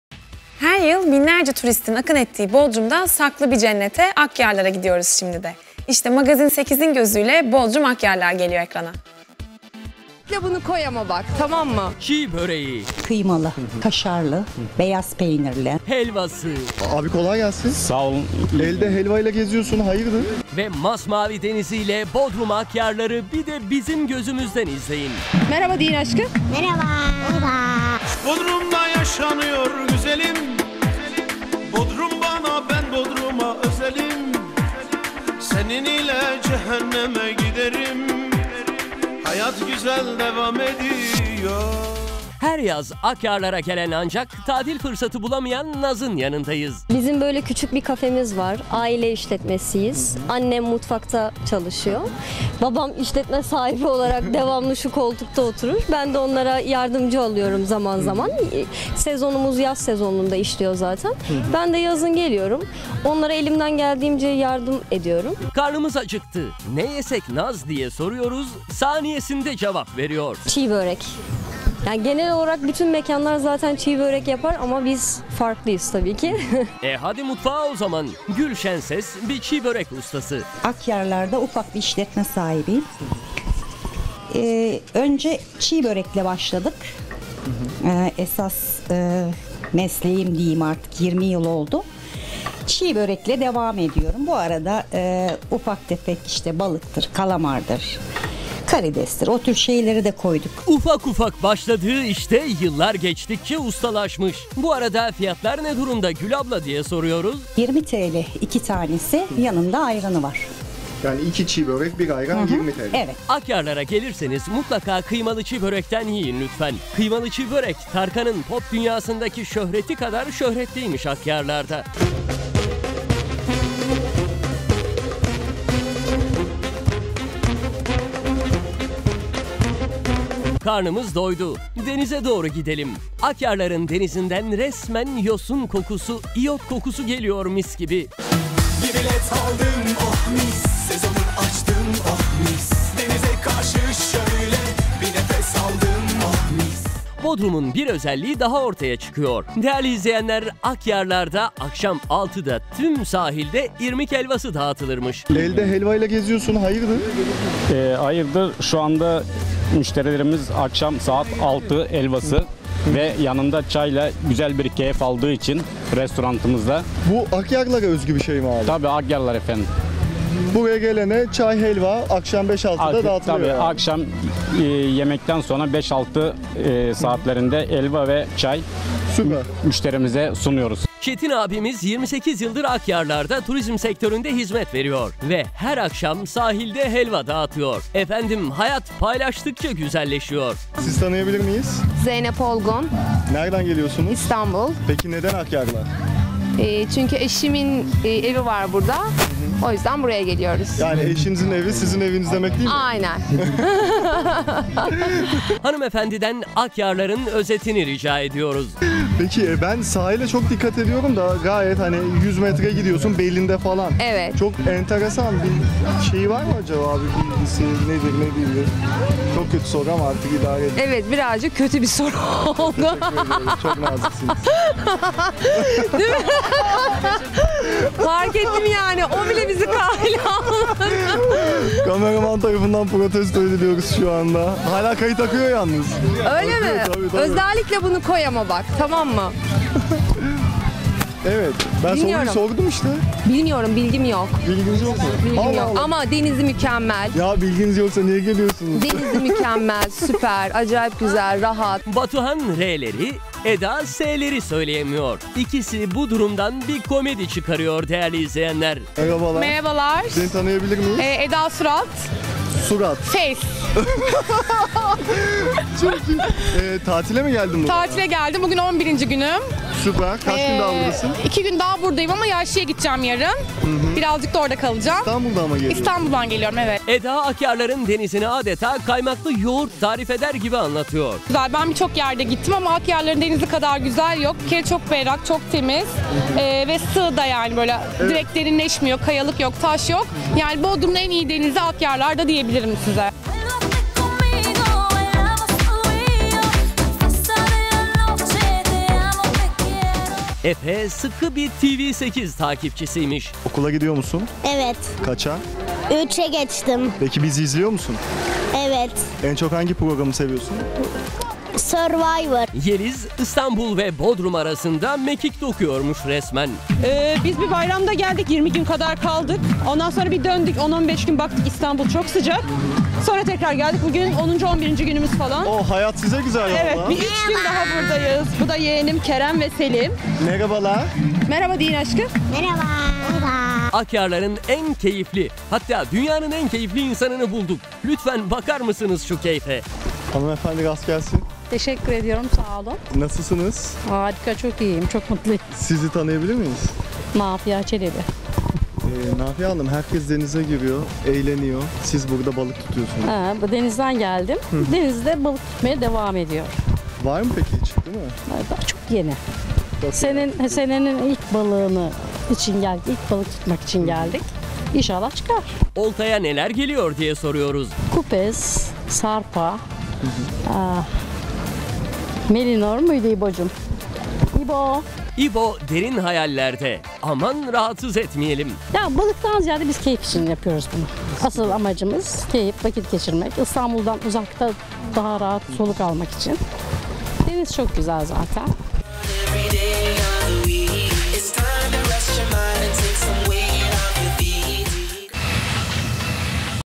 Tatil Planı Yapanlar İçin Bodrum Rehberi
Türkiye'nin turizm başkenti Bodrum'a nasıl gidilir, nereler görülmelidir, nerede kalınır, ne yenilir, ne içilir? Bodrum rehberi.